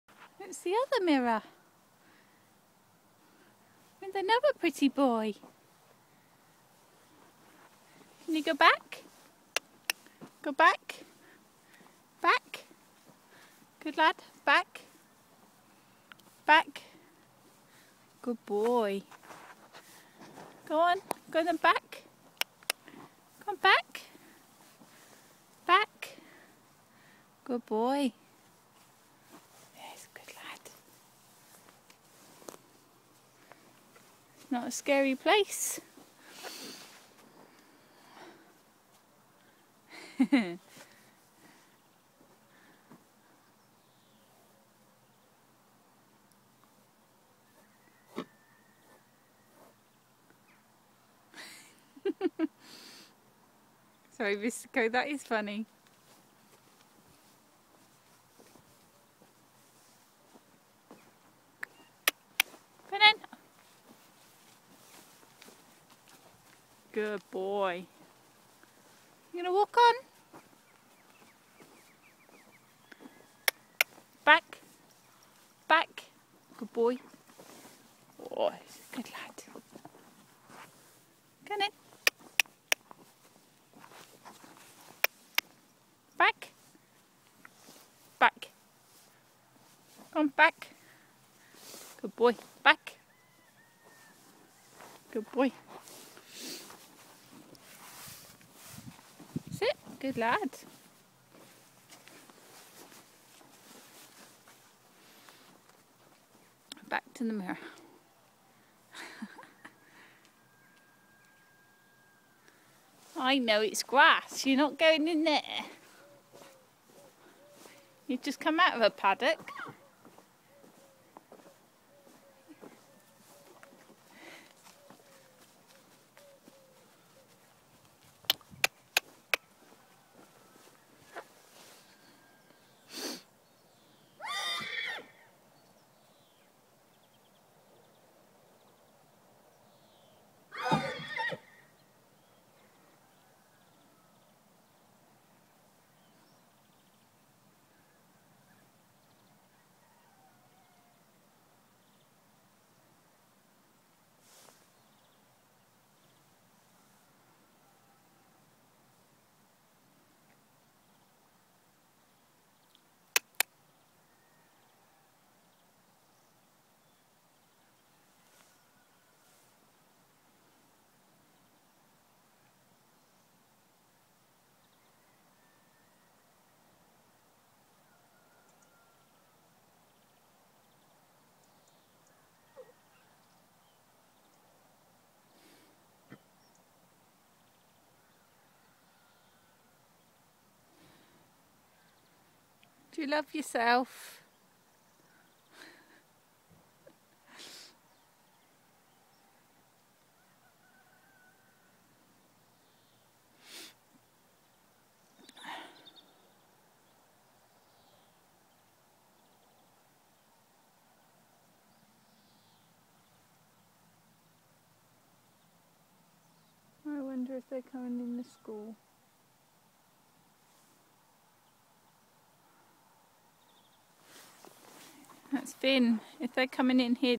Mystico looking in the Mirror
My PRE since moving to a new yard has had issues with the school its not been a fun place to be so i let him off today in it to walk around. Video shot 29th Jan 2012